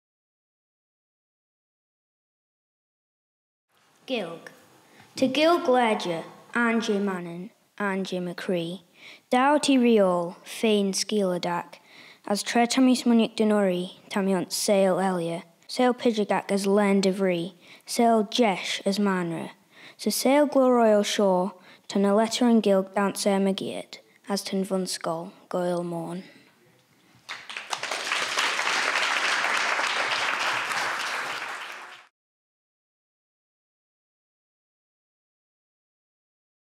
'Gaelg': A poem in Manx Gaelic by Matilda
A poem in Manx Gaelic, on the importance of learning the language of the Isle of Man.
The poem was written and performed by Matilda.

This is a part of the “Manks Concert” held at the Peel Centenary Centre on 24 May 2019, the anniversary of the birth of Sophia Morrison.

The event was organised by the Manx Branch of the Celtic Congress
The Celtic Congress was founded in 1902 in order to promote the knowledge, use, and appreciation of the languages and cultures of the six Celtic nations. The Manx branch today continues to perpetuate the culture, ideals, and languages of the Celtic peoples, and to maintain an intellectual contact and close cooperation with the other Celtic communities.
We are very grateful to the Celtic Congress for agreeing to our recording and releasing the concert here.